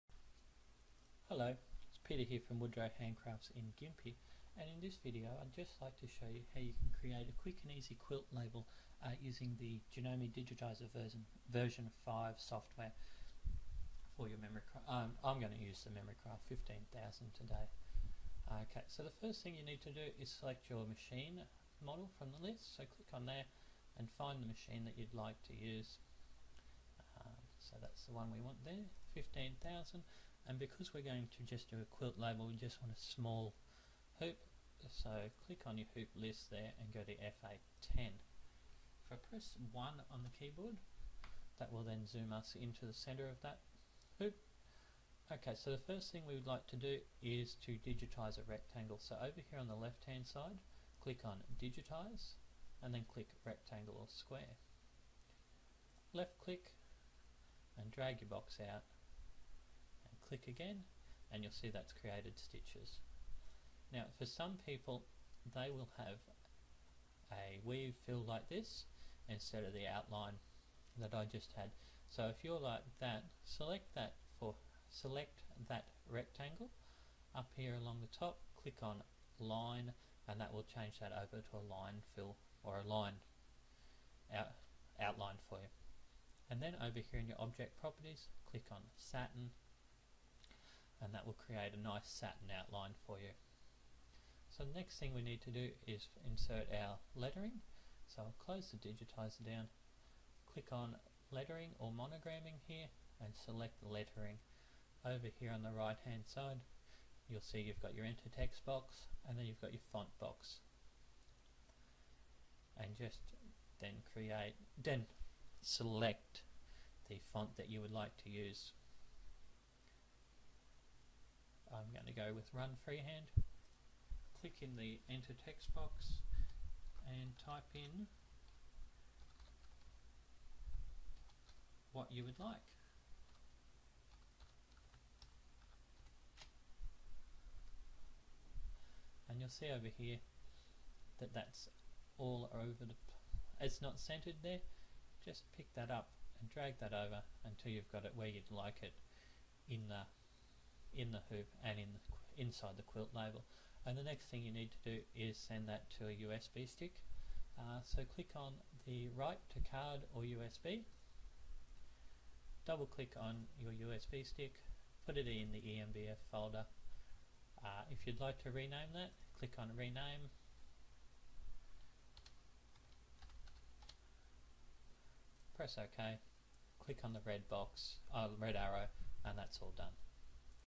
Quick and Easy Quilt Label: Janome Digitizer V5
Create a quick and easy quilt label using the Digitizer V5 software